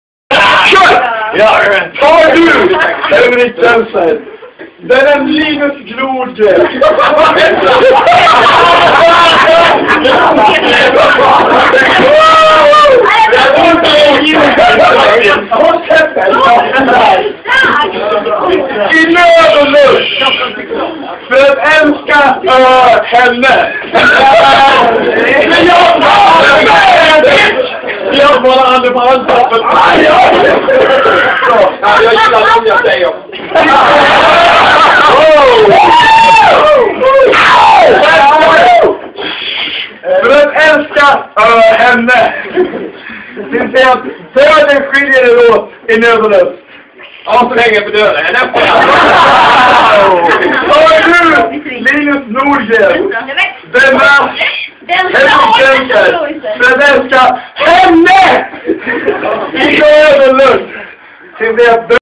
Skhumt Bröllop
The belowed Henric Denzler is getting married, finally, after more than one year in captivity - or engagement... Priceless...